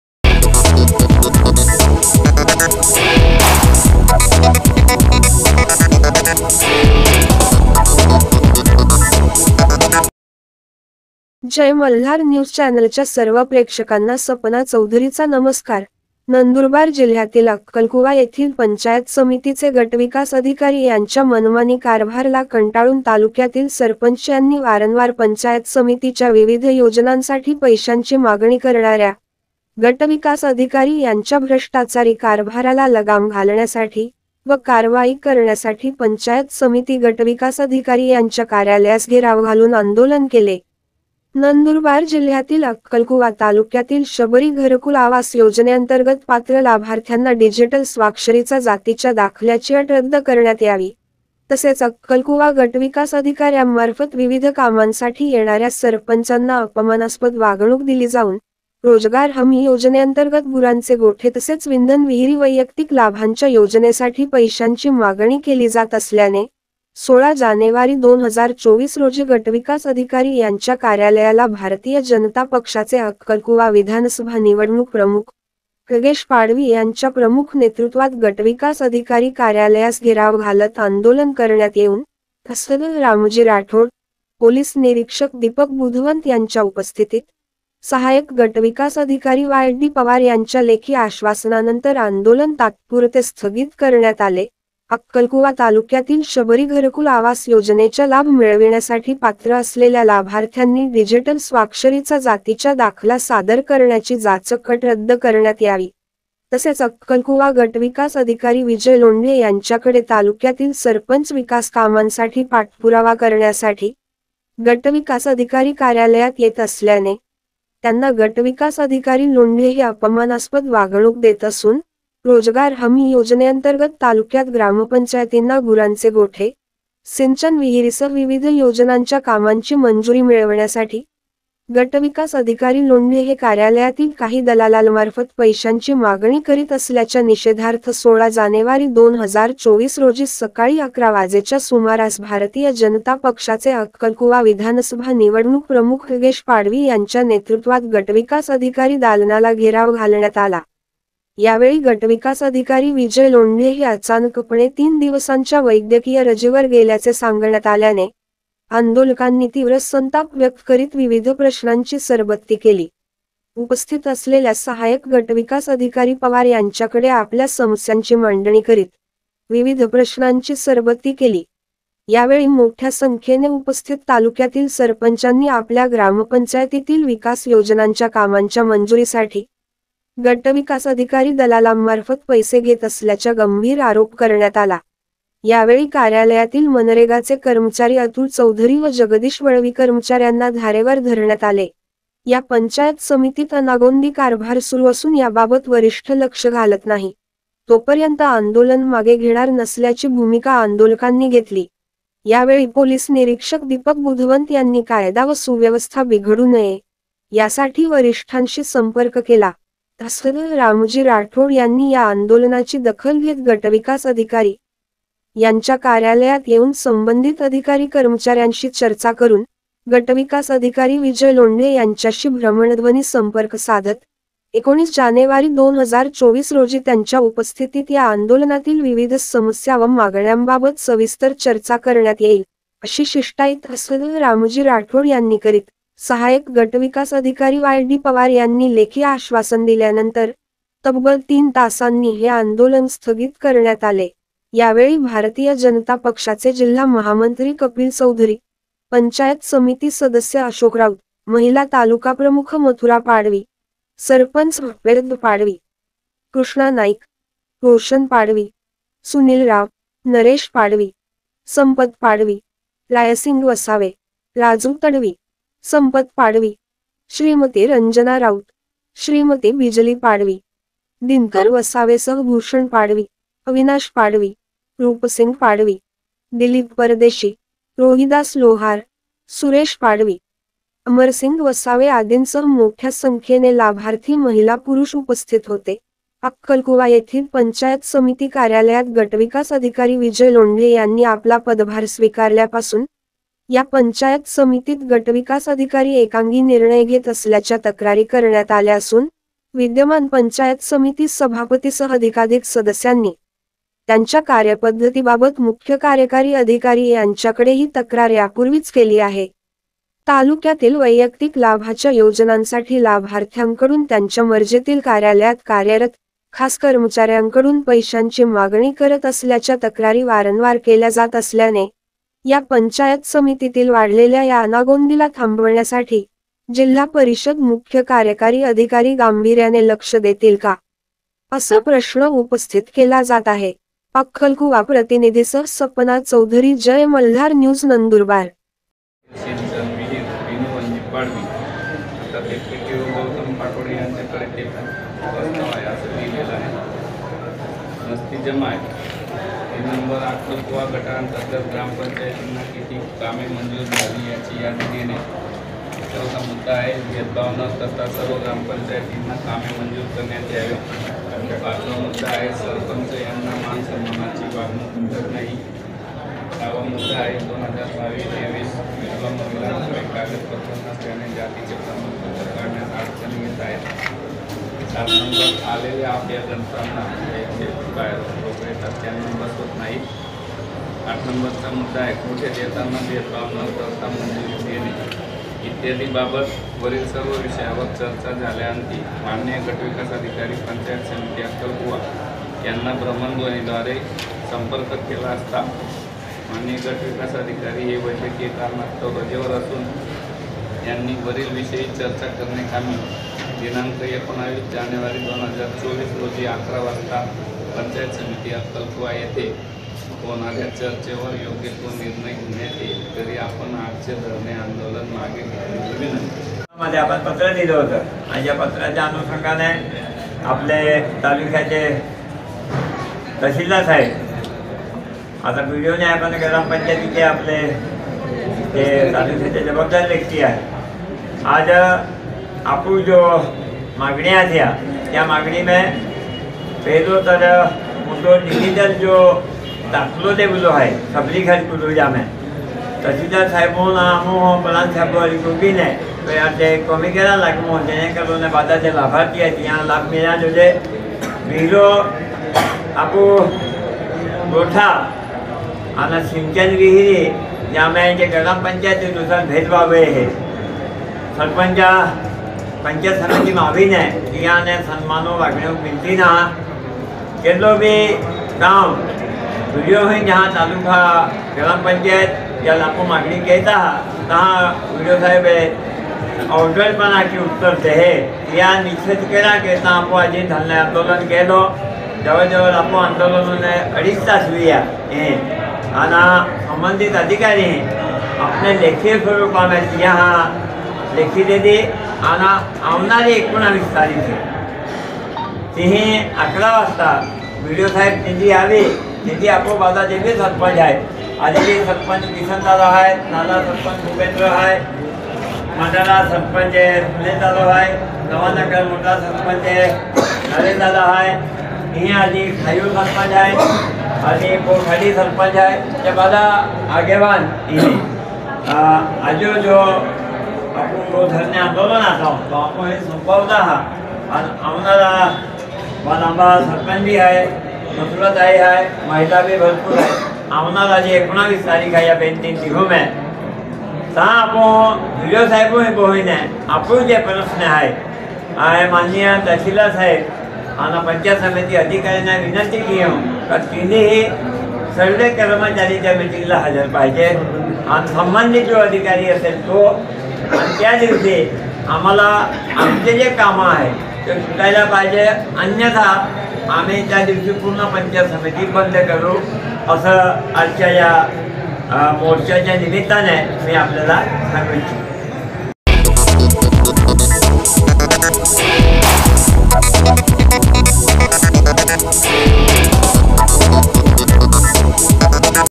अक्कलकुवा पं.स.गटविकास अधिकरी यांच्या भ्रष्टाचारास लगाम घालण्यासाठी गटविकास कार्यालयास घेराव,आंदोलन
नंदुरबार जिल्ह्यातील अक्कलकुवा येथील पंचायत समितीचे गटविकास अधिकारी यांच्या मनमानी कारभारला कंटाळून तालुक्यातील सरपंच यांनी वारंवार पंचायत समितीच्या विविध योजनांसाठी पैशांची मागणी करणाऱ्या, गटविकास अधिकारी यांच्या भ्रष्टाचारी कारभाराला लगाम घालण्यासाठी, व कारवाई करण्यासाठी पंचायत समिती गटविकास अधिकारी यांच्या कार्यालयास घेराव घालून आंदोलन केले. नंदूरबार जिल्ह्यातील अक्कलकुवा तालुक्यातील शबरी घरकुल आवास योजनेअंतर्गत पात्र लाभार्थ्यांना डिजिटल स्वाक्षरीचा जातीच्या दाखल्याची अट रद्द करण्यात यावी, तसेच अक्कलकुवा गटविकास अधिकाऱ्यांमार्फत विविध कामांसाठी येणाऱ्या सरपंचांना अपमानास्पद वागणूक दिली जाऊन, रोजगार हमी योजनेअंतर्गत गुरांचे गोठे तसेच विंधन विहिरी  वैयक्तिक लाभांच्या योजनेसाठी पैशांची मागणी केली जात असल्याने, 16 जानेवारी 2024 रोजी गटविकास अधिकारी यांच्या कार्यालयाला भारतीय जनता पक्षाचे अक्कलकुवा विधानसभा निवडणूक प्रमुख, नागेश पाडवी यांच्या प्रमुख नेतृत्वात गटविकास अधिकारी कार्यालयास घेराव घालत आंदोलन करण्यात येऊन, तहसीलदार रामजी राठोड, पोलीस निरीक्षक दीपक बुधवंत यांच्या उपस्थितीत, सहाय्यक गटविकास अधिकारी  वाय डी पवार यांच्या लेखी आश्वासनानंतर आंदोलन तात्पुरते स्थगित करण्यात आले, अक्कलकुवा तालुक्यातील शबरी घरकुल आवास योजनेच्या लाभ मिळविण्यासाठी पात्र असलेल्या लाभार्थ्यांनी डिजिटल स्वाक्षरीचा जातीच्या दाखला सादर करण्याची जाचक अट रद्द करण्यात यावी, तसेच अक्कलकुवा गटविकास अधिकारी विजय लोंढे यांच्याकडे तालुक्यातील सरपंच विकास कामांसाठी पाठपुरावा करण्यासाठी, गटविकास अधिकारी कार्यालयात येत असल्याने, त्यांना गटविकास अधिकारी लोंढे हे अपमानास्पद वागणूक देत असून, रोजगार हमी योजनेअंतर्गत तालुक्यात ग्रामपंचायतींना गुरांचे गोठे, सिंचन विहिरीसह विविध योजनांच्या कामांची मंजुरी मिळवण्यासाठी, गटविकास अधिकारी लोंढे हे कार्यालयातील काही दलालालमार्फत पैशांची मागणी करीत असल्याच्या निषेधार्थ 16 जानेवारी 2024 रोजी सकाळी 11 वाजेच्या सुमारास भारतीय जनता पक्षाचे अक्कलकुवा विधानसभा निवडणूक प्रमुख नागेश पाडवी यांच्या नेतृत्वात गटविकास अधिकारी दालनाला घेराव घालण्यात आला, यावेळी गटविकास अधिकारी विजय लोंढे हे अचानकपणे तीन दिवसांच्या वैद्यकीय रजेवर गेल्याचे सांगण्यात आल्याने, आंदोलकांनी तीव्र संताप व्यक्त करीत विविध प्रश्नांची सरबत्ती केली, उपस्थित असलेल्या सहाय्यक गटविकास अधिकारी पवार यांच्याकडे आपल्या समस्यांची मांडणी करीत, विविध प्रश्नांची सरबत्ती केली, यावेळी मोठ्या संख्येने उपस्थित तालुक्यातील सरपंचांनी आपल्या ग्रामपंचायतीतील विकास योजनांच्या कामांच्या मंजुरीसाठी, गटविकास अधिकारी दलालांमार्फत पैसे घेत असल्याच्या गंभीर आरोप करण्यात आला, यावेळी कार्यालयातील मनरेगाचे कर्मचारी अतुल चौधरी व जगदीश वळवी कर्मचाऱ्यांना धारेवर धरण्यात आले, या पंचायत समितीत अनागोंदी कारभार सुरू असून याबाबत वरिष्ठ लक्ष घालत नाही, तोपर्यंत आंदोलन मागे घेणार नसल्याची भूमिका आंदोलकांनी घेतली, यावेळी पोलीस निरीक्षक दीपक बुधवंत यांनी कायदा व सुव्यवस्था बिघडू नये, यासाठी वरिष्ठांशी संपर्क केला, तहसीलदार रामजी राठोड यांनी या आंदोलनाची दखल घेत गटविकास अधिकारी, यांच्या कार्यालयात येऊन संबंधित अधिकारी कर्मचाऱ्यांशी चर्चा करून, गटविकास अधिकारी विजय लोंढे यांच्याशी भ्रमणध्वनी संपर्क साधत, 19 जानेवारी 2024 रोजी त्यांच्या उपस्थितीत या आंदोलनातील विविध समस्या व मागण्यांबाबत सविस्तर चर्चा  करण्यात येईल, अशी शिष्टाई तहसीलदार रामजी राठोड यांनी करीत, सहाय्यक गटविकास अधिकारी वायडी पवार यांनी लेखी आश्वासन दिल्यानंतर, तब्बल तीन तासांनी हे आंदोलन स्थगित करण्यात आले, यावेळी भारतीय जनता पक्षाचे जिल्हा महामंत्री कपिल चौधरी, पंचायत समिती सदस्य अशोक राऊत, महिला तालुकाप्रमुख मथुरा पाडवी, सरपंच भूपेंद्र पाडवी, कृष्णा नाईक, रोशन पाडवी, सुनील राव, नरेश पाडवी, संपत पाडवी, रायसिंग वसावे, राजू तडवी, संपत पाडवी, श्रीमती रंजना राऊत, श्रीमती बिजली पाडवी, दिनकर वसावेसह भूषण पाडवी, अविनाश पाडवी, रूपसिंग पाडवी, दिलीप परदेशी, रोहिदास लोहार, सुरेश पाडवी, अमरसिंग वसावे आदींसह मोठ्या संख्येने लाभार्थी महिला पुरुष उपस्थित होते, अक्कलकुवा येथील पंचायत समिती कार्यालयात गटविकास अधिकारी विजय लोंढे यांनी आपला पदभार स्वीकारल्यापासून, या पंचायत समितीत गटविकास अधिकारी एकांगी निर्णय घेत असल्याच्या तक्रारी करण्यात आल्या असून, विद्यमान पंचायत समिती सभापतीसह अधिकाधिक सदस्यांनी, त्यांच्या कार्यपद्धतीबाबत मुख्य कार्यकारी अधिकारी यांच्याकडेही तक्रार यापूर्वीच केली आहे,  तालुक्यातील वैयक्तिक लाभाच्या योजनांसाठी लाभार्थ्यांकडून त्यांच्या मर्जीतील कार्यालयात कार्यरत, खास कर्मचाऱ्यांकडून पैशांची मागणी करत असल्याच्या तक्रारी वारंवार केल्या जात असल्याने, या पंचायत समितीतील वाढलेल्या या अनागोंदीला थांबवण्यासाठी, जिल्हा परिषद मुख्य कार्यकारी अधिकारी गांभीर्याने लक्ष देतील का ? असा प्रश्न उपस्थित केला जात आहे.